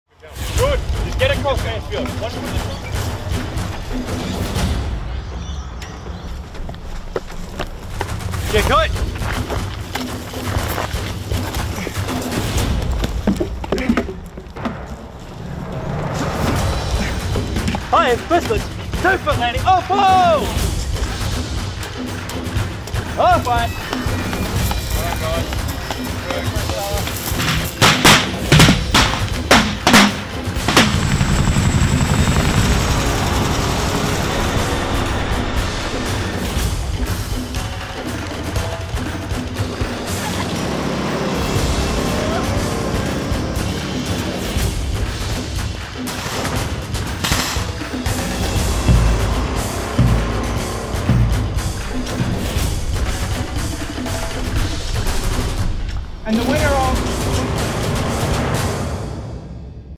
ADF | Australian Army 2023 3rd Brigade Section Competition
Congrats to 1RAR for winning the 2023 Brigade Section Competition! 👏

The 3rd Brigade Section Competition was held at Lavarack Barracks, Mount Stuart Training Area and Tully Training Area, QLD, during October. The competition involved eight sections from within the Brigade testing themselves over a series of military skills stands. 💪

Stands included the obstacle course, marksmanship serials, casevac medical training, military flotation, section attack, section defense, communications, navigation, obstacle deduction and all arms call for fire with artillery assets.

Well done to all!